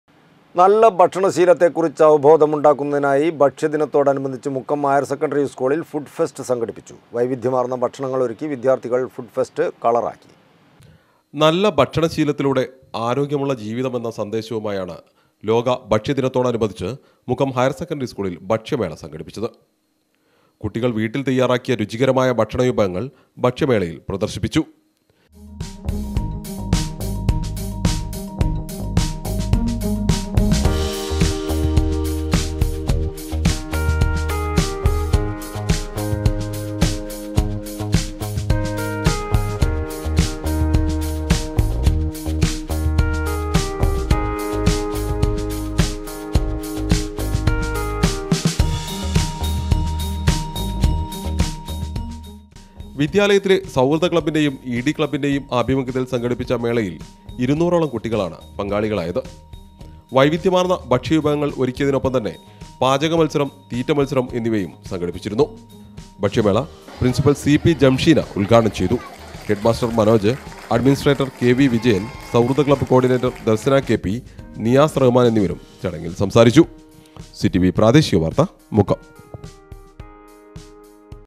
മുക്കം ഹയർ സെക്കന്ററി സ്കൂളിൽ ഫുഡ് ഫെസ്റ്റ് സംഘടിപ്പിച്ചു
നല്ല ഭക്ഷണ ശീലത്തെ കുറിച്ച് അവബോധമുണ്ടാക്കുന്നതിനായി ഭക്ഷ്യദിനത്തോടനുബന്ധിച്ച് മുക്കം ഹയർ സെക്കന്ററി സ്കൂളിൽ ഫുഡ് ഫെസ്റ്റ് സംഘടിപ്പിച്ചു